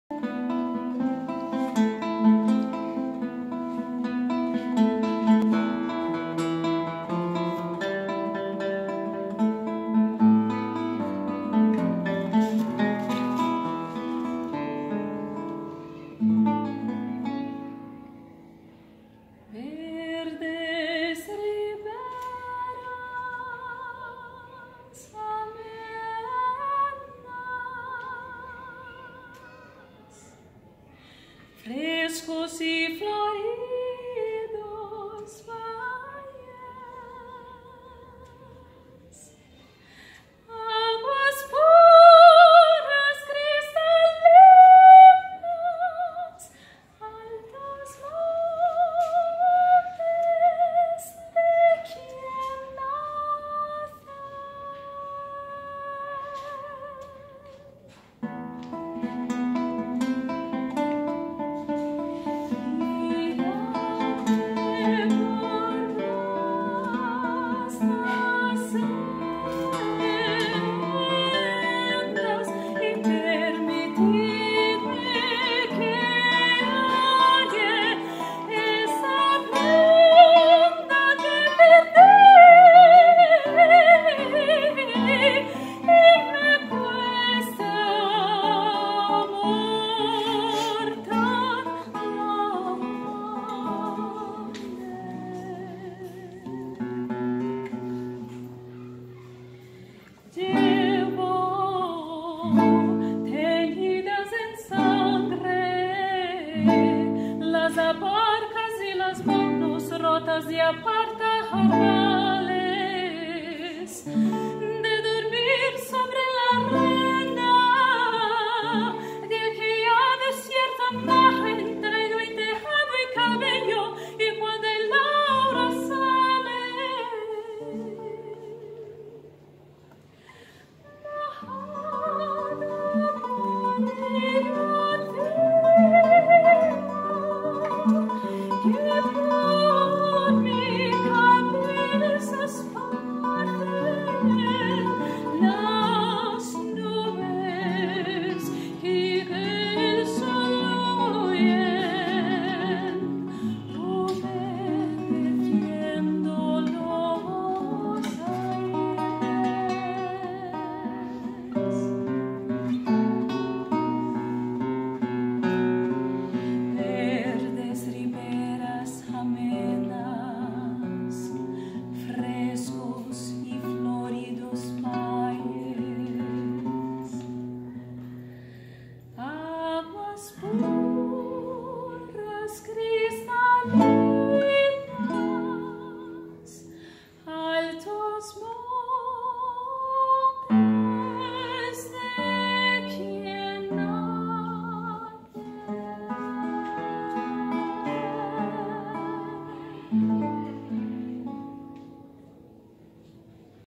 O Som da Bocaina: Aline Tolon e Max Riccio - Parte 2
Apresentação do Duo Iara no projeto "O Som da Bocaina", no Solar Aguiar Valim, no dia 09/03/2019. Aline Tolon (voz) e Max Riccio (violão)